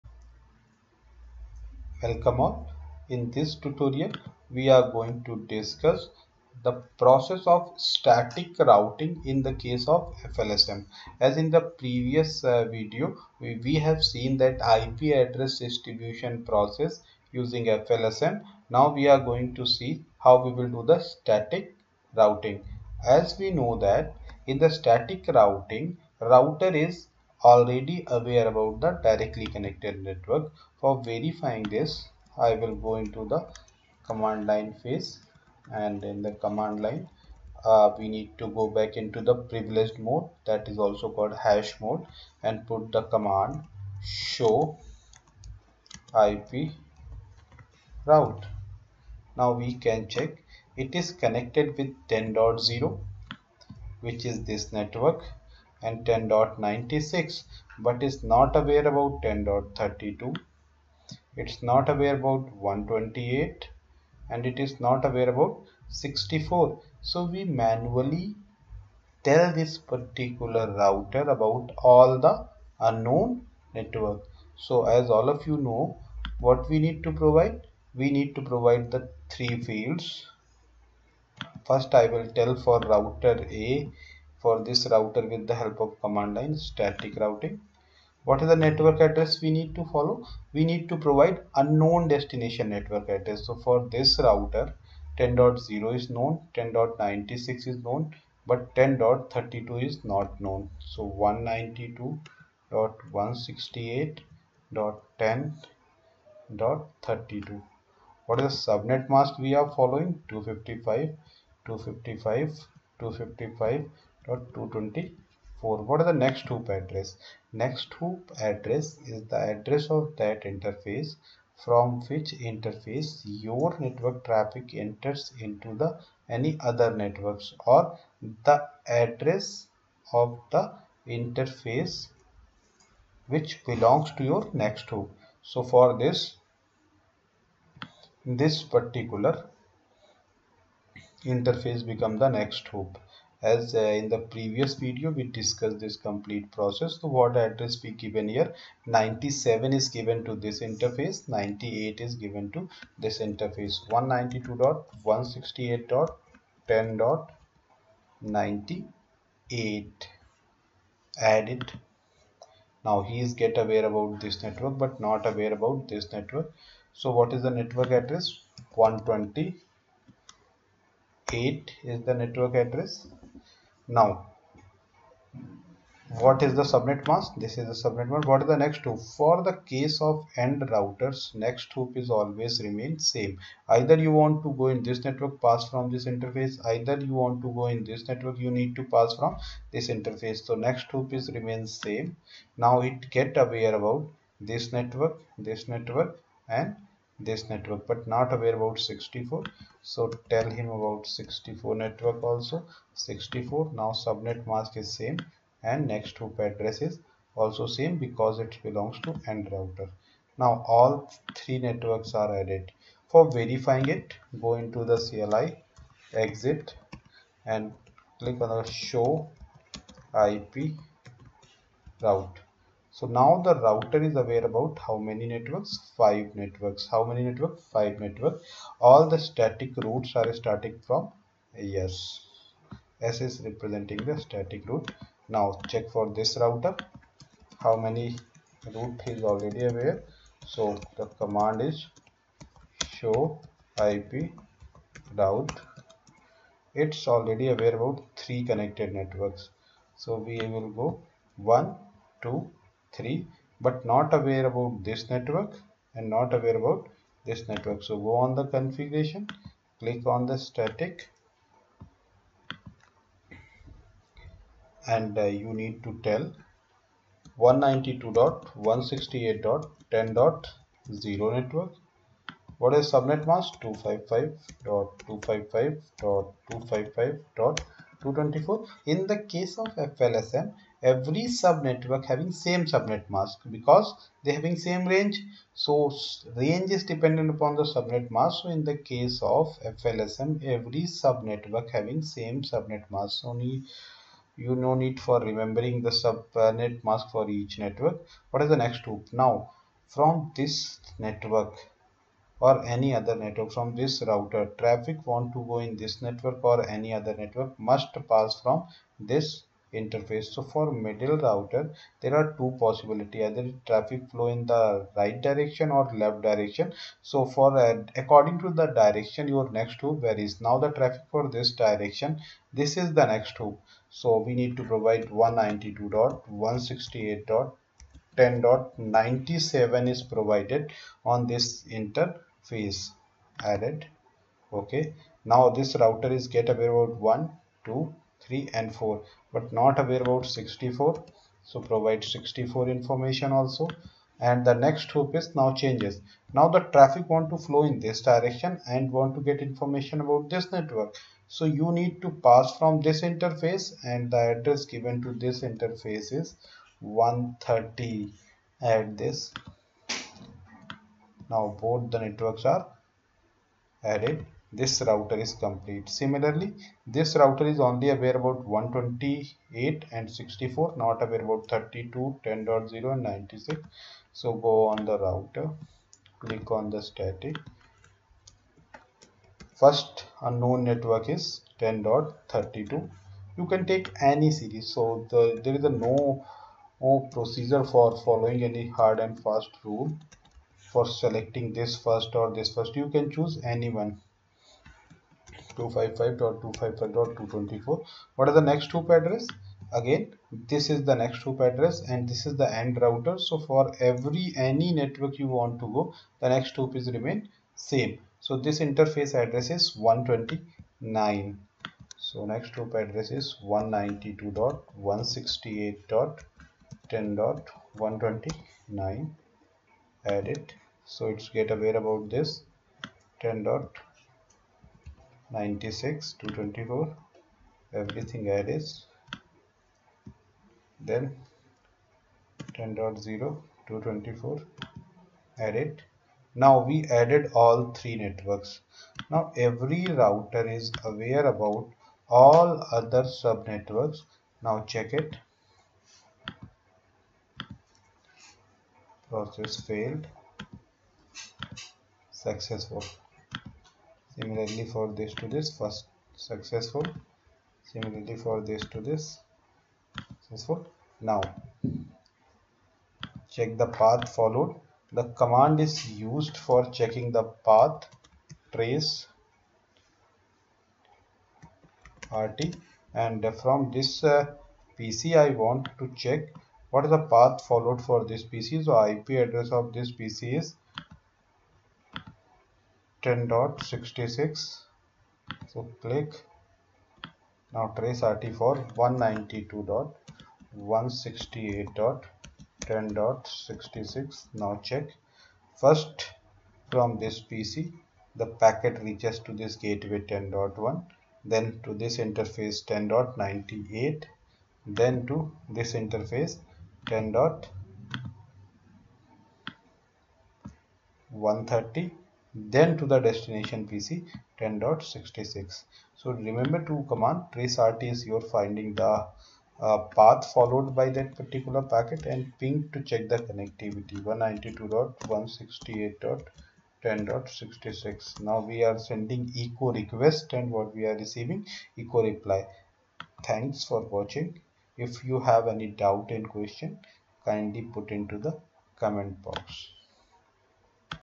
Networking Lab-12 | FLSM STATIC ROUTING using 3 Routers| Cisco Packet Tracker | Engineering Tutorial
After FLSM Addressing, THIS VIDEO DEMONSTRATES about FLSM Static Routing using three ROUTERS.
*We Recommend watching our previous Videos on SUBNETTING and FLSM Addressing.

Put your queries and comments in the comment section and